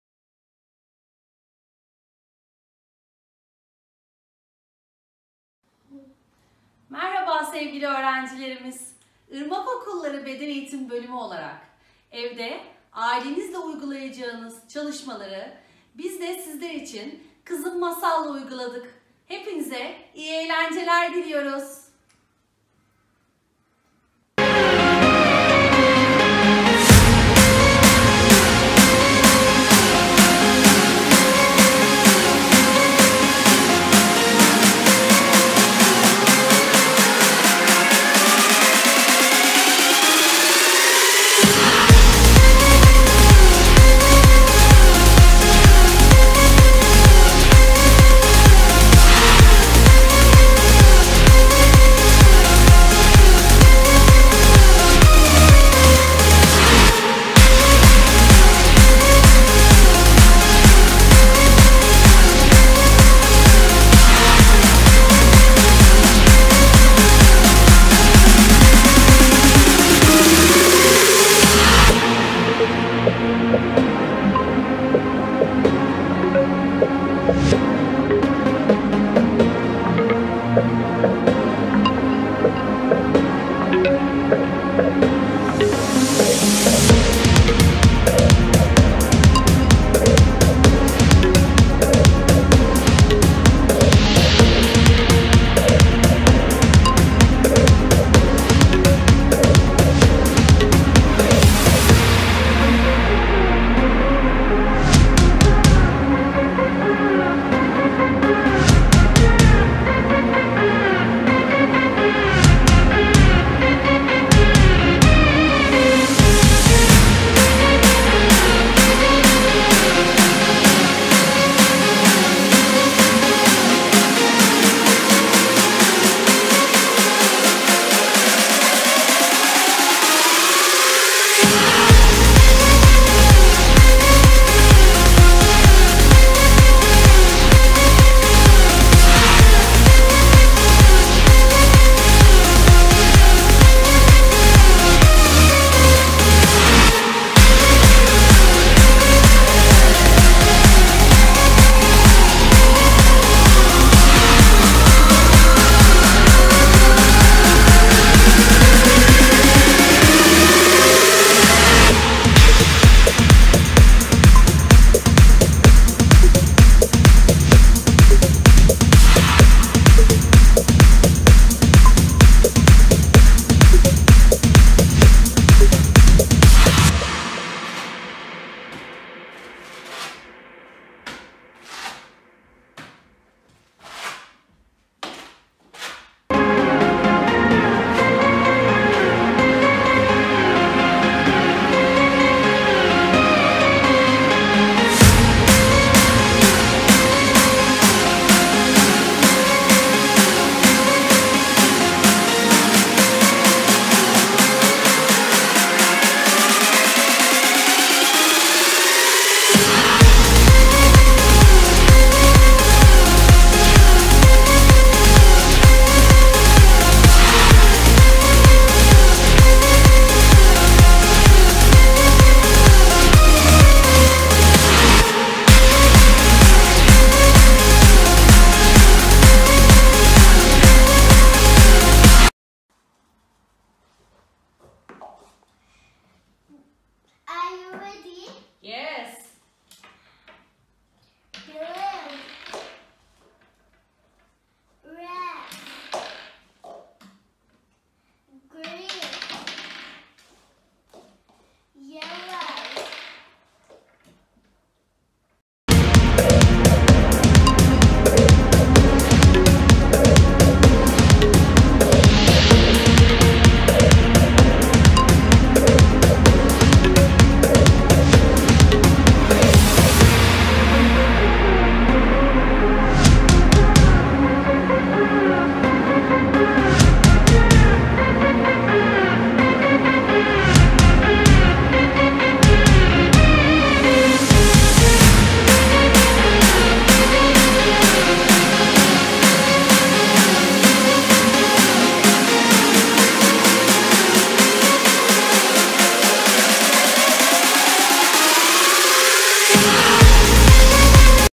Indoor Activities P.E
Hazırladığımız bu çalışmadaki temel amacımız; sağlığımızı korumak için evlerimizde bulunduğumuz  bu günlerde, sizleri biraz da olsa hareketli bir yaşama teşvik etmektir. Evleriniz de bulunan materyallerle bu egzersizleri oynayabilir, çocuklarınızla keyifli zamanlar geçirebilirsiniz.